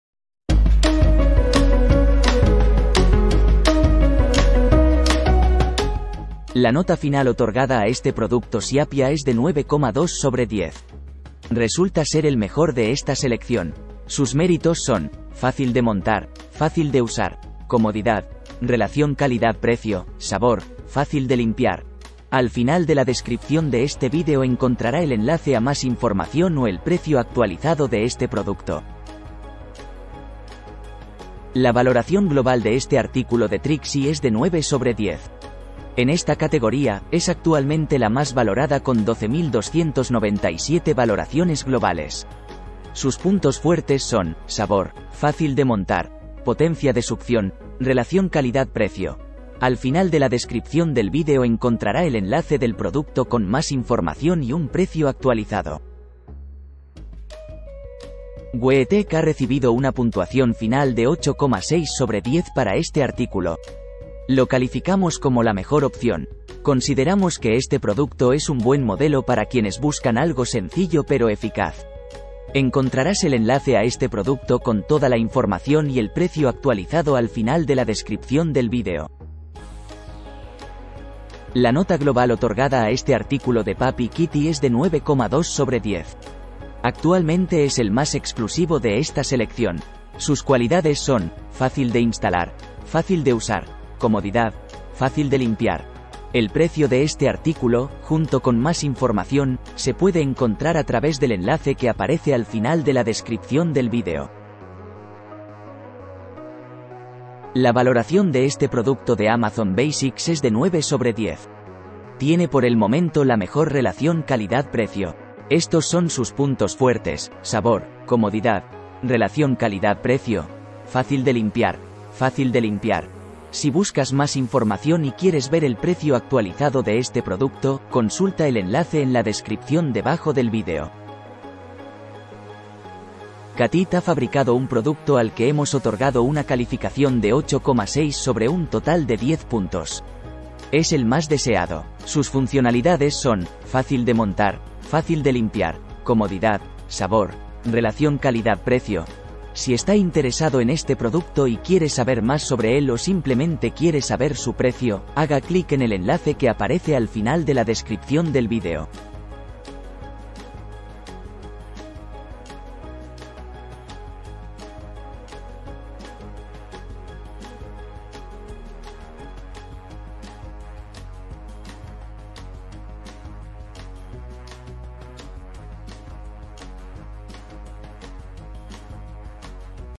Comedero para gatos - Top 6 - (CuálElijo)
📣 Enlaces de 'Comedero para gatos' en este vídeo:

✅ LO MEJOR: XIAPIA Dispensador de Agua Automático para Mascotas, Comeder...

✅ MÁS VOTADO: Trixie Cat Activity Fun Board, 5 Opciones, 30x40 cm

✅ NUESTRA ELECCIÓN: UEETEK Bowl tazón de fuente del animal doméstico gato alimen...

✅ LO MÁS EXCLUSIVO: PUPPY KITTY Comedero automático para Perros y Gatos, Aliment...

✅ MEJOR CALIDAD PRECIO: Amazon Basics - Espacio de actividades y alimentación para m...

Índice:
00:00 - Introducción
00:05 - XIAPIA Dispensador de Agua Automático para Mascotas, Comeder...
00:30 - Trixie Cat Activity Fun Board, 5 Opciones, 30x40 cm
00:57 - UEETEK Bowl tazón de fuente del animal doméstico gato alimen...
01:20 - PUPPY KITTY Comedero automático para Perros y Gatos, Aliment...
01:46 - Amazon Basics - Espacio de actividades y alimentación para m...
02:12 - Catit Excavadora Senses 2.0